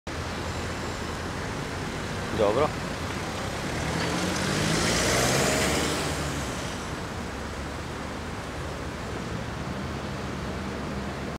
Пуцњава у Београду
Пуцњава у Београду: Мушкарац пуцао у радницу пекаре, па покушао да изврши самоубиство!
Snimio: Novosti